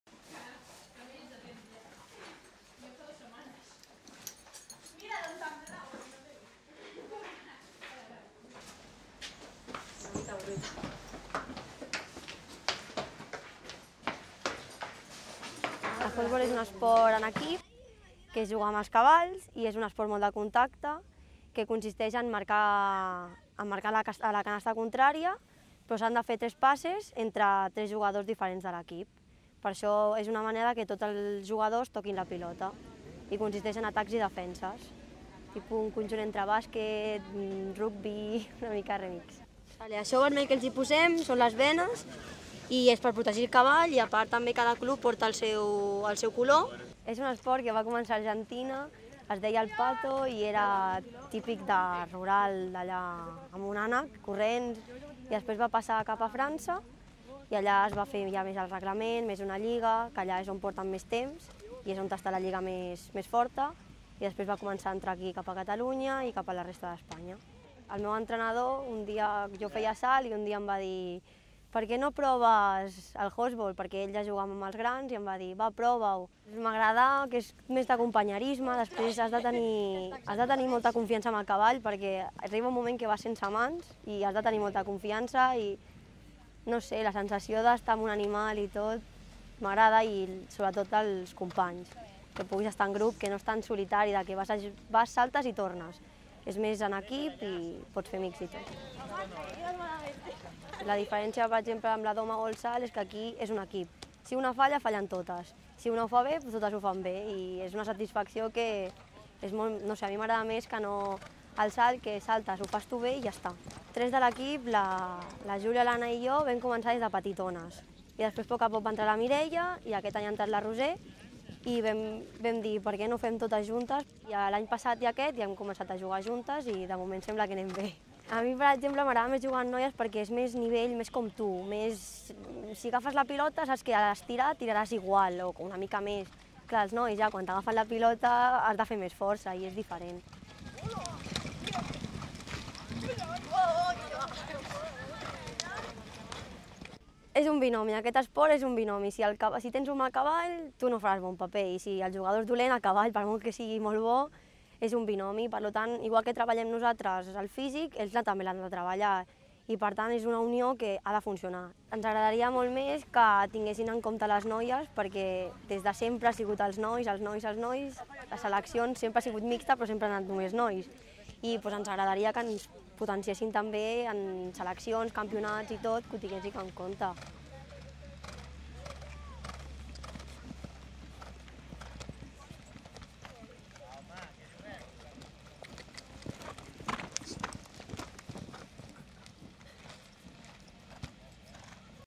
Cardedeu té un dels millors equips de horseball de noies
La Irene Valencia juga des de ben petita a horseball a un dels 6 únics equips femenins que hi ha a Catalunya, el Club d'Equitació Esportiva de Cardedeu. Fa dos anys el grup de noies que jugaven amb els nois van decidir fer el seu propi equip i ara competeixen a la lliga femenina de catalunya, on lideren la classificació. Ella va començar a jugar a horseball després de fer molts anys de doma clàssica. A Careddeu hi ha el millor equip de Horseball del món, segons la federació internacional de horseball.